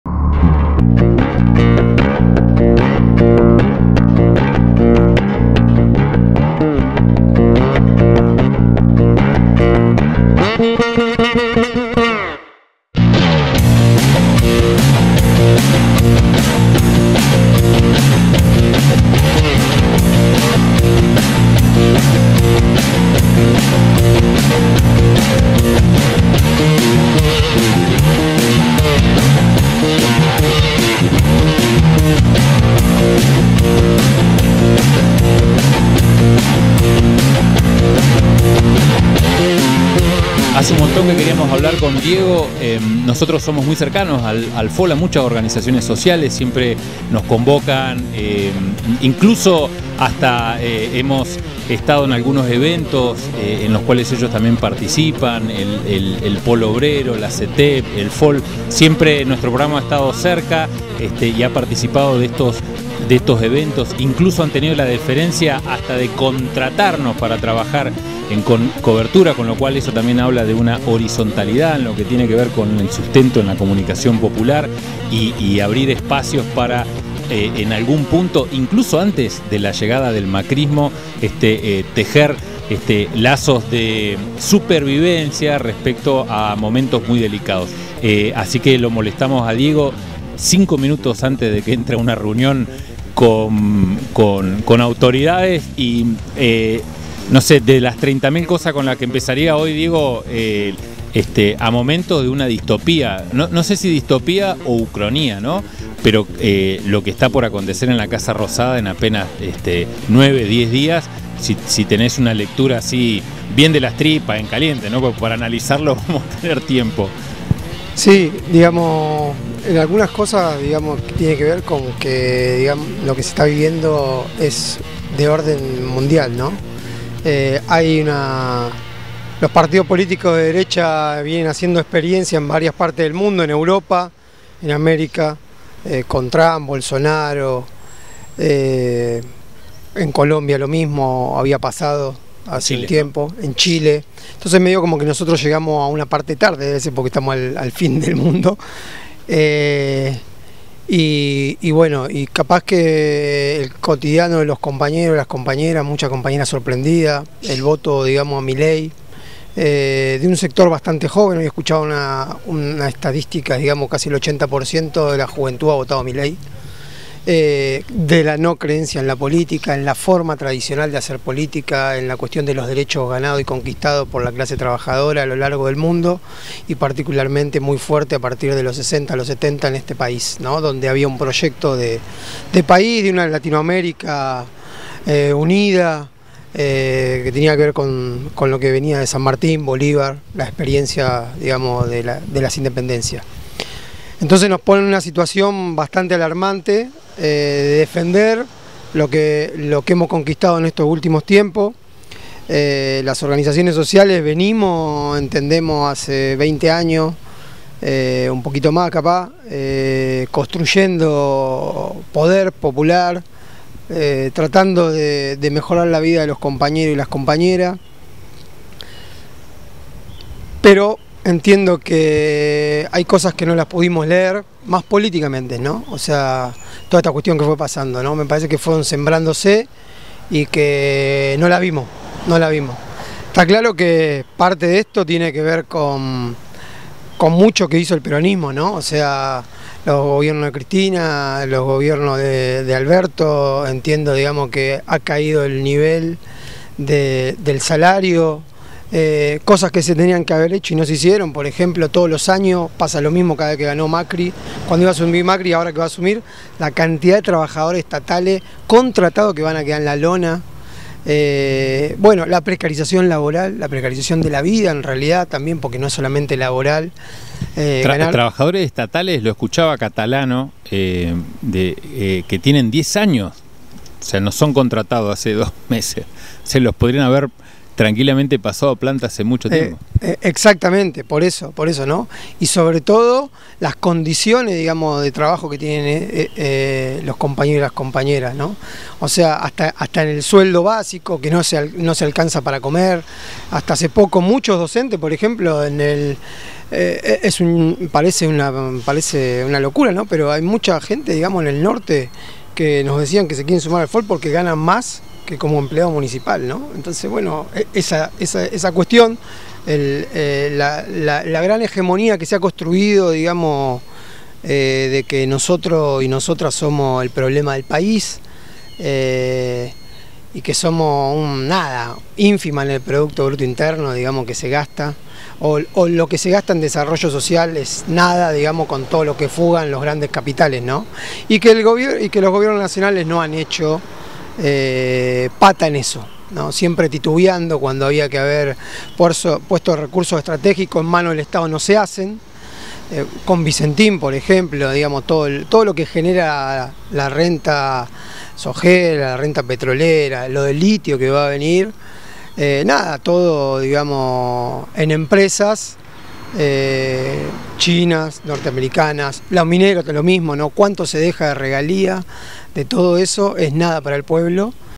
Diego Mauro FOL "Hace 20 años que estamos construyendo Poder Popular"
Diego Mauro es referente del Frente de Organizaciones en Lucha de Neuquén sobre las primeras medidas del gobierno nacional y cómo se preparan para intervenir frente al ajuste neoliberal